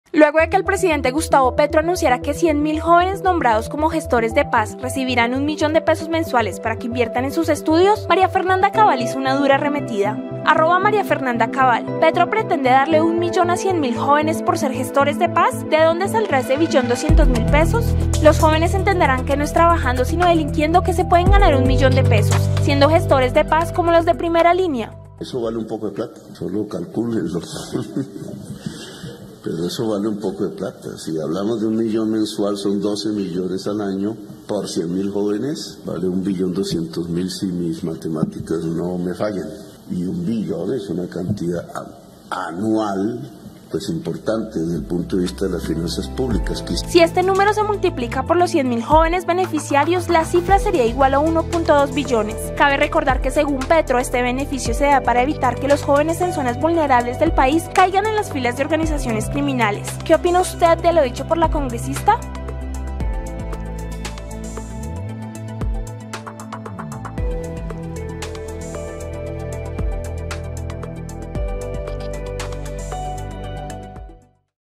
Petro y la dura arremetida de María Fernanda por gestores de paz
Aquí le contamos los detalles de la dura arremetida de María Fernanda Cabal porque gestores de paz recibirán un millón de pesos mensuales.

¿Qué opina de lo dicho por la congresista?